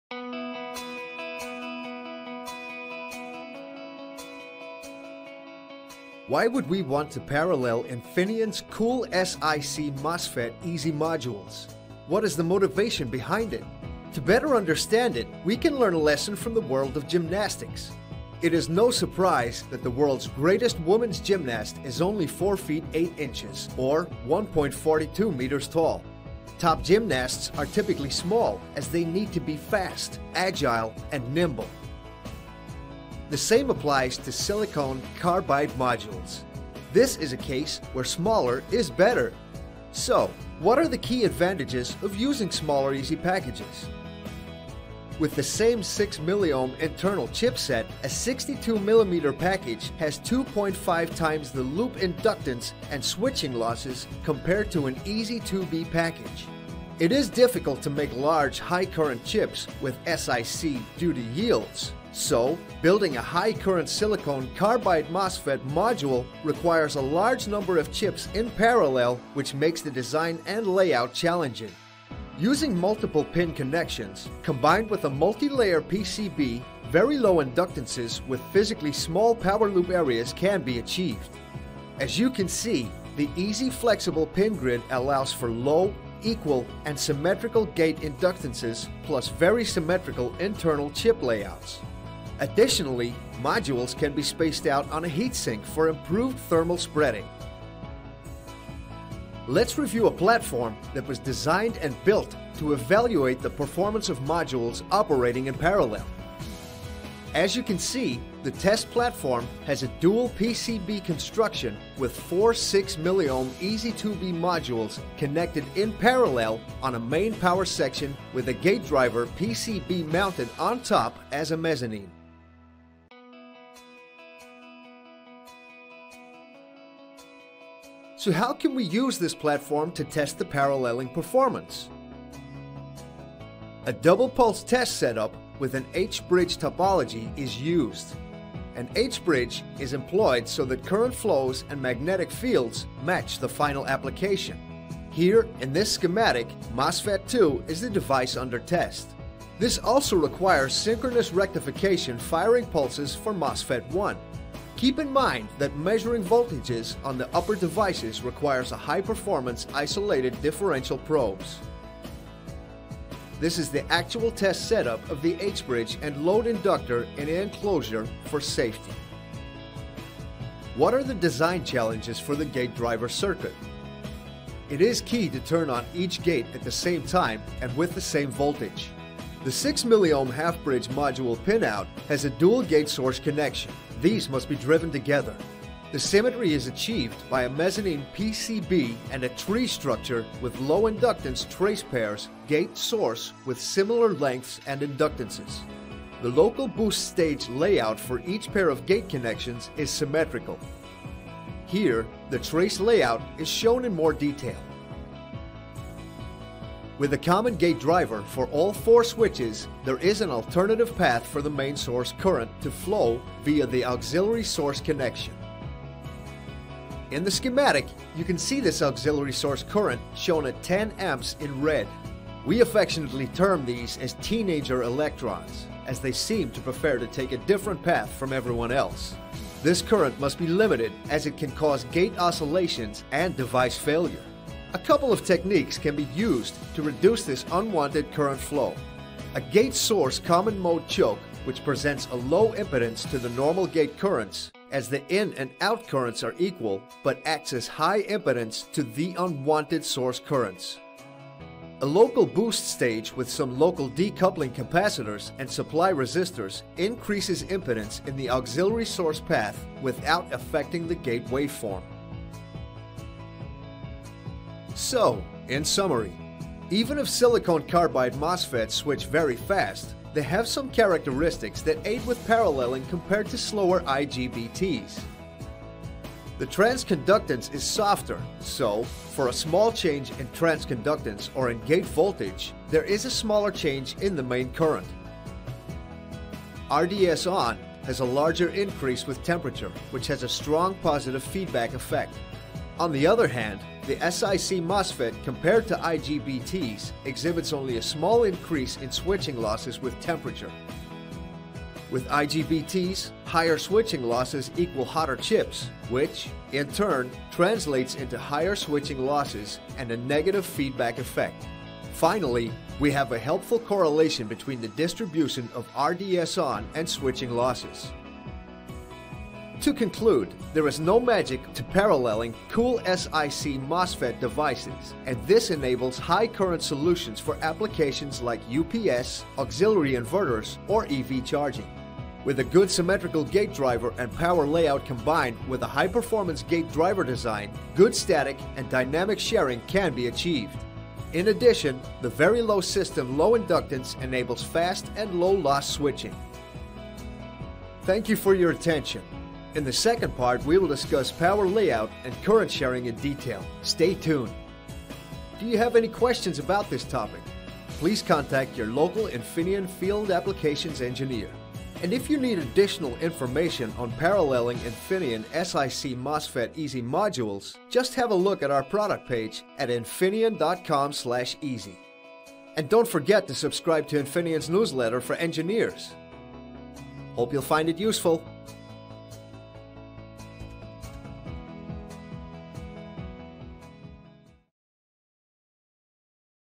Optimizing SiC MOSFET Module Paralleling for Enhanced Power Efficiency | Infineon
Explore the dynamic realm of SiC MOSFET module paralleling and unlock the potential for much faster device switching speeds.

This eLearning presents a comprehensive understanding of the rationale behind paralleling SiC MOSFET modules, addressing key challenges, and providing solutions for gate driver and power layout design.

 Additionally, delve into the strategies for optimized system loop inductance to minimize switching losses. Join us in uncovering the transformative potential of SiC MOSFET module paralleling, and gain valuable insights into enhancing power efficiency.

Stay informed and empowered with essential knowledge about SiC MOSFET technology and its pivotal role in optimizing power module paralleling.

00:00 What is the motivation?
00:40 What are the key advantages?
01:38 How can testing be performed?
02:50 Challenges?
04:24 Summary
05:19 Conclusion
05:47 Outro